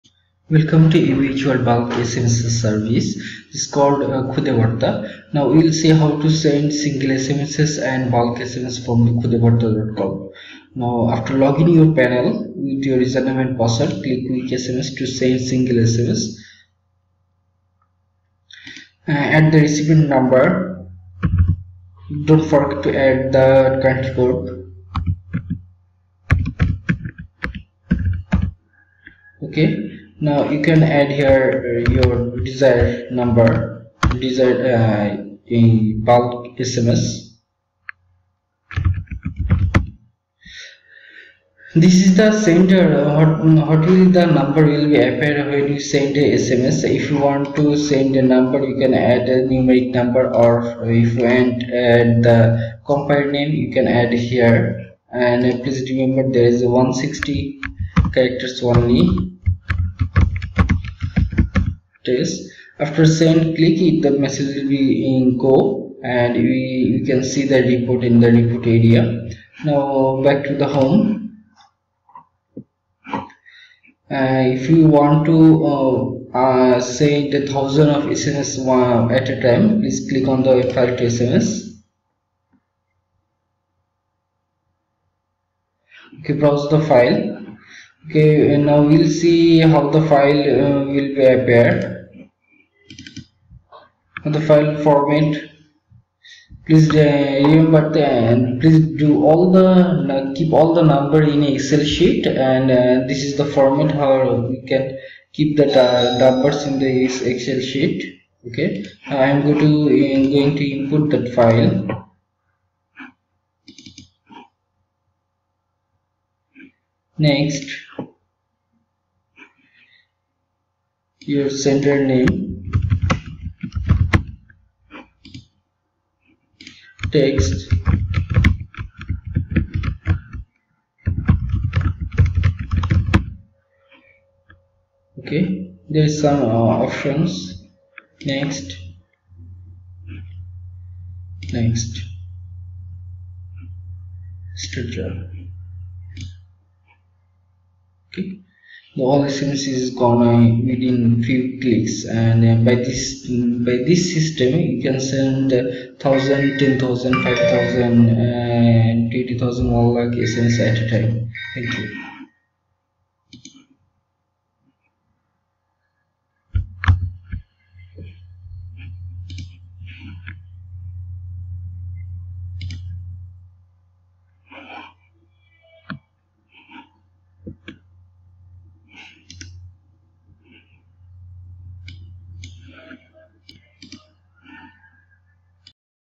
How to send Bulk SMS
ABH Wold has bulk SMS service called Khudebarta. In this video you will see how to send SMS using khudebarta.